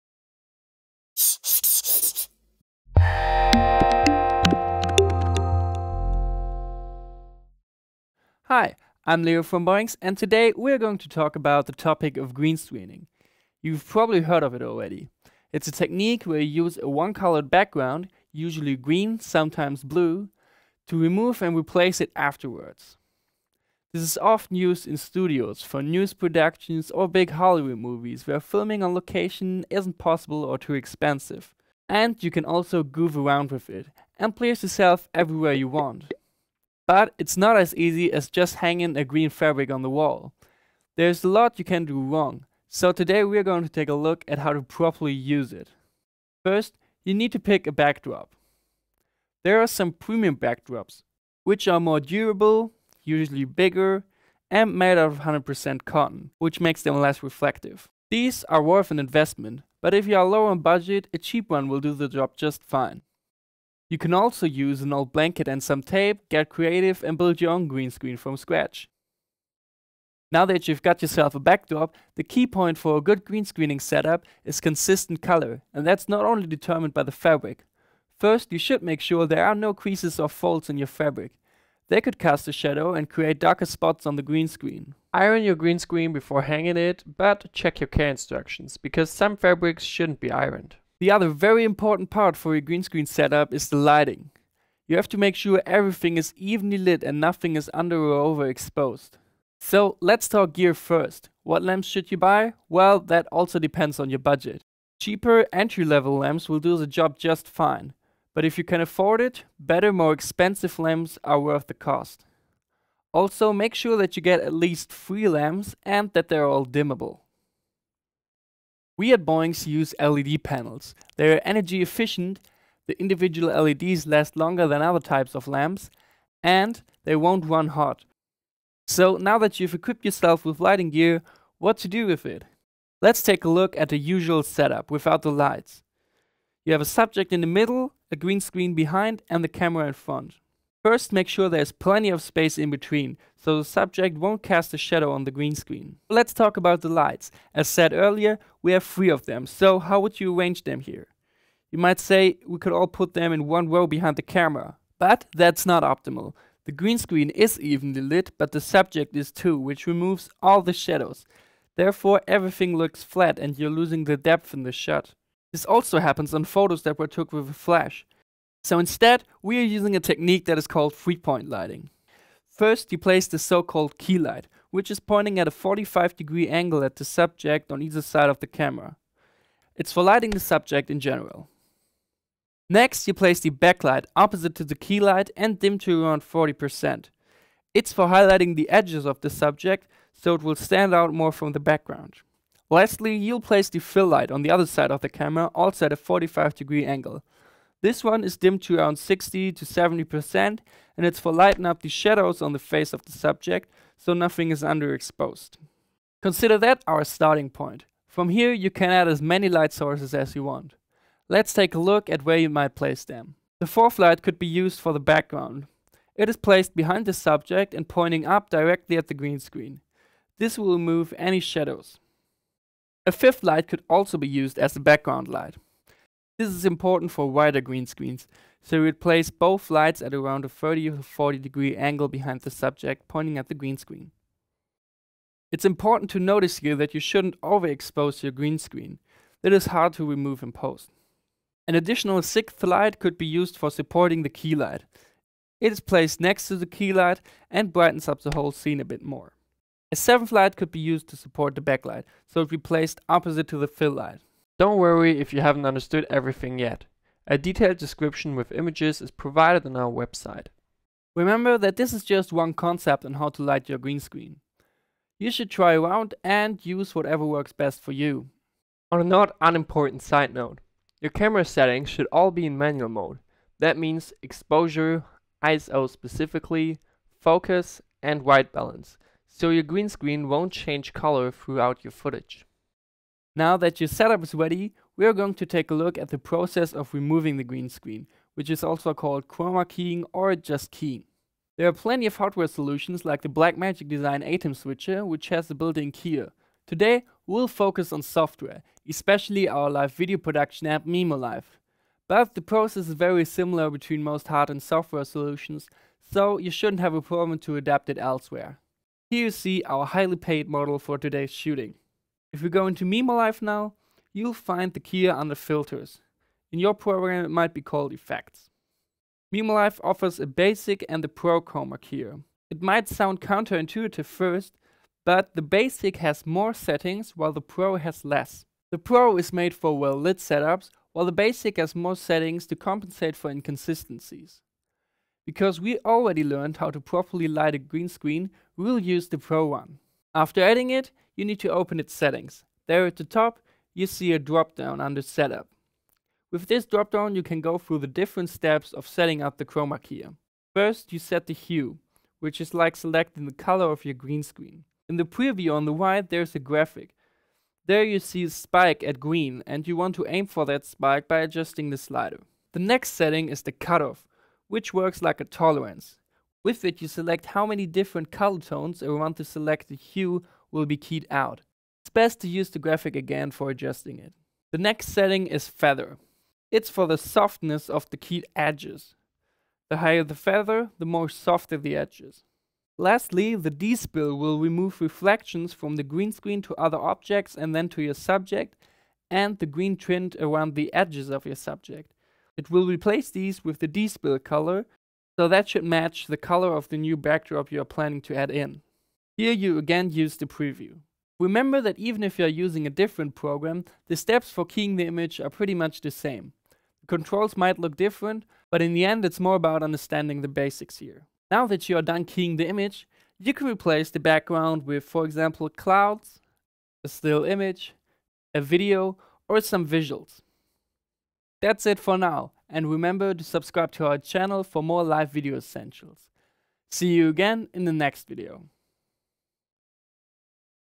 How to build a Green Screen in a home setting | LSE E01
Live Streaming Essentials - E01
Leo explains in this video how to add transparency to your video by using a green screen.
He goes through each step of how to set up and properly light the green screen. Additionally, you will learn how to configure the chroma key filters in mimoLive to get the transparency effect in your live video.

This video is part of his video series “Live Streaming Essentials” in which Leo talks about the fundamentals of live video topics to help you get started and grow beyond the beginner level.

0:00 Intro
0:10 About Green Screens
0:46 Green backdrop
1:30 Lighting a the green screen
3:52 Studio Light Setup
4:56 Green Screen in mimoLive